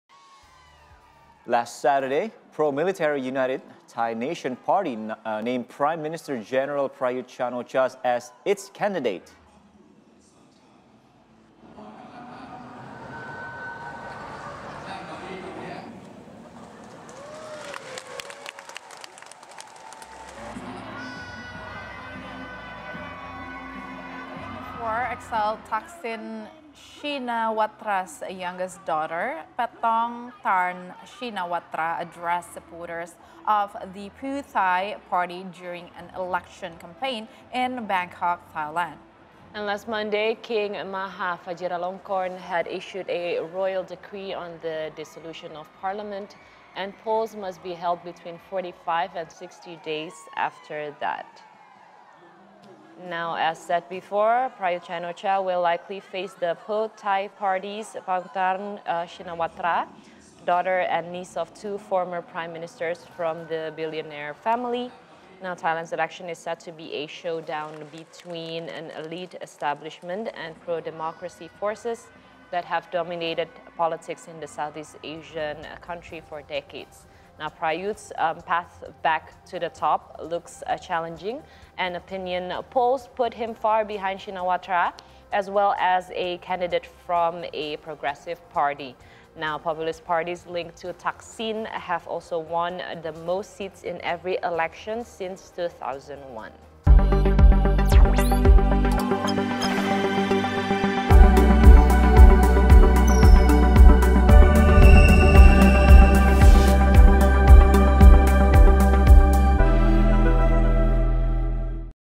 Gen. Prayut Chan-Ocha To Run As Candidate In The Elections
Serangkaian momen politik dari United Thai Nation Party dan Pheu Thai Party telah berlangsung di Thailand menjelang pemilihan umum pada 14 Mei mendatang. Senin lalu (20/3) Raja Maha Vajiralongkorn telah mengeluarkan dekrit kerajaan tentang pembubaran parlemen dan pemungutan suara harus diadakan antara 45 dan 60 hari setelah itu.
A series of political moments from United Thai Nation Party and Pheu Thai Party have taken place in Thailand, ahead of the general election on May the 14th. Last Monday (3/20) King Maha Vajiralongkorn had issued a royal decree on the dissolution of parliament and polls must be held between 45 and 60 days after that.